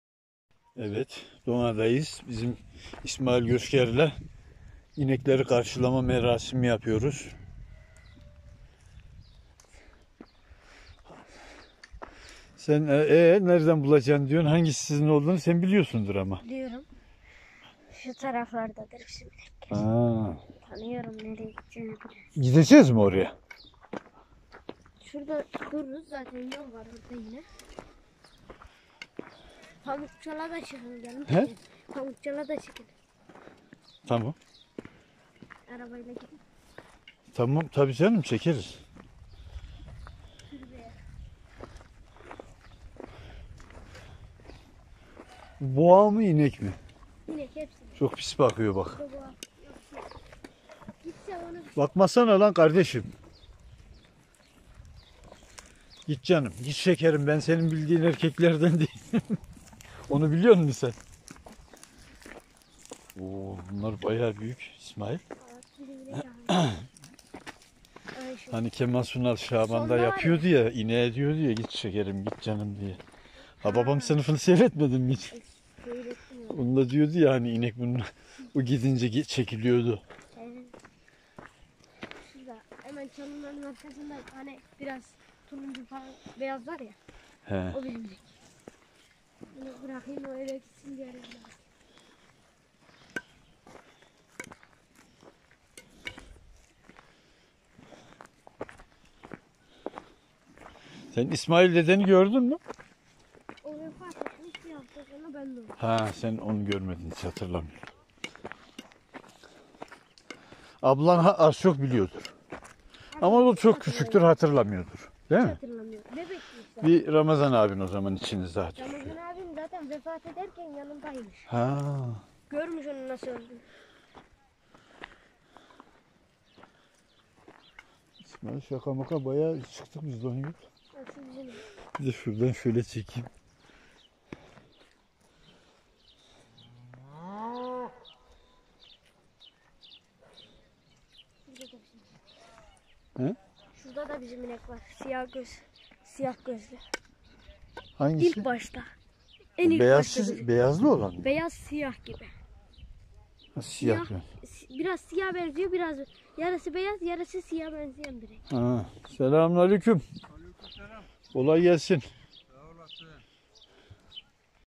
KÖY Gezisi küçük çoban İsmail ile (KÜÇÜK DONA KÖYÜ)
MUHTEŞEM BİR DUYGU ADETA KÖKLERİMİZE DÖNDÜK